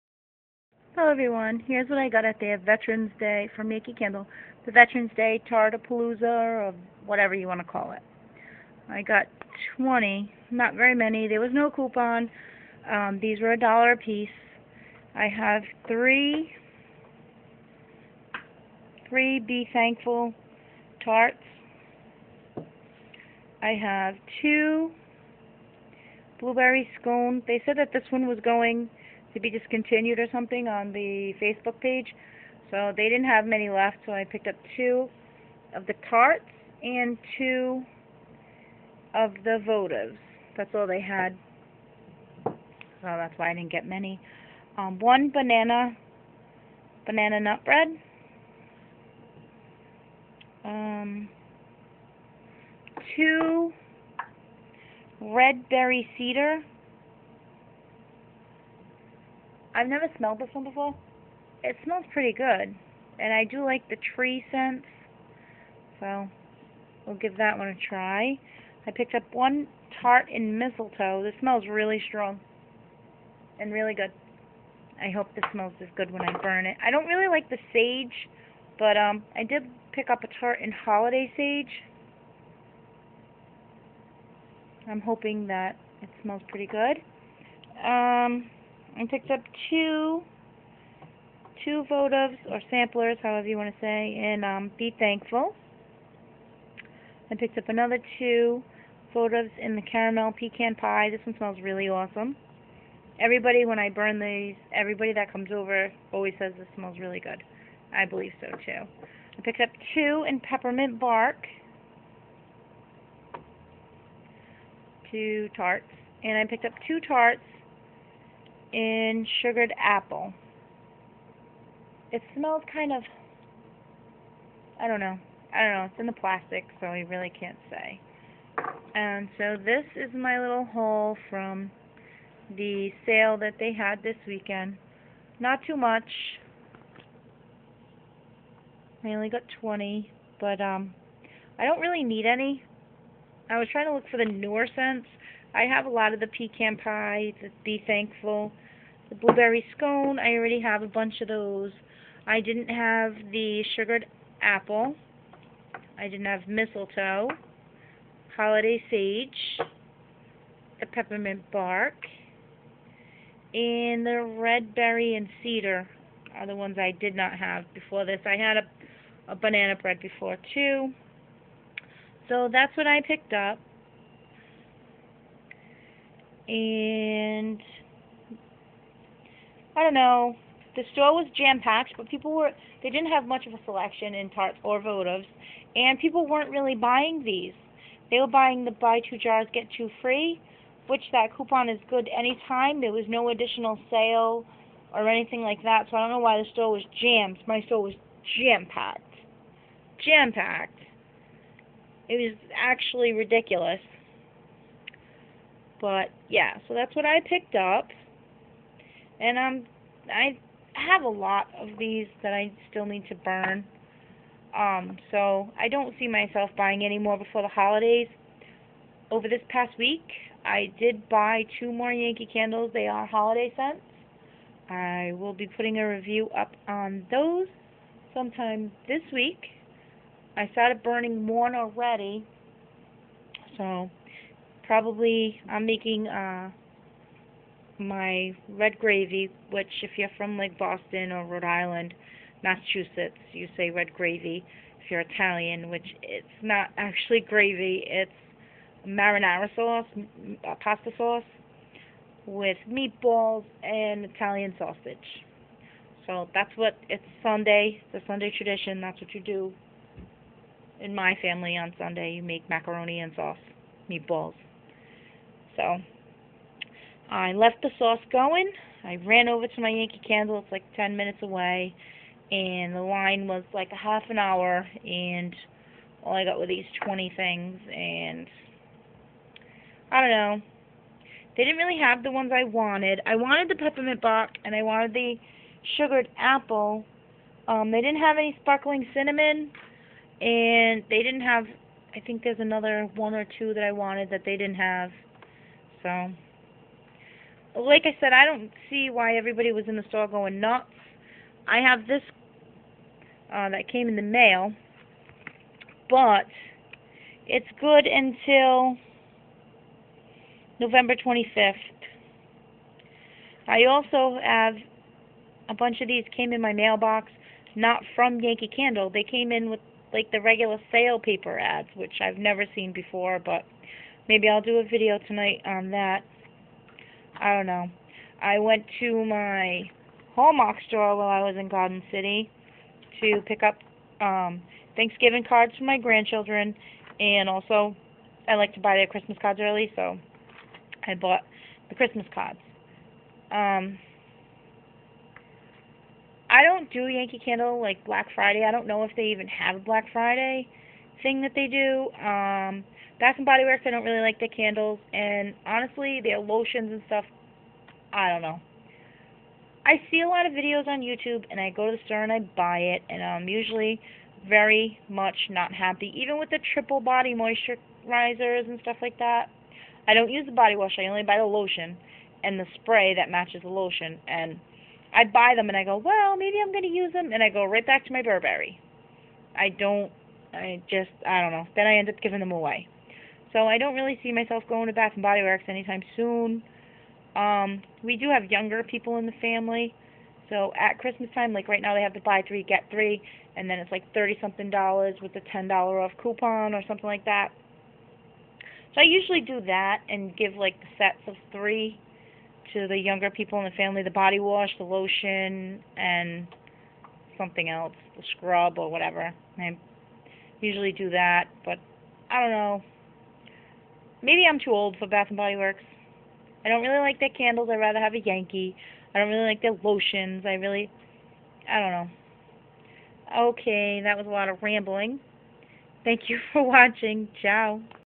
Yankee Candle $1 Tart Sale
I went the tart sale they were having this weekend $1 tart & votive. I only picked up 20.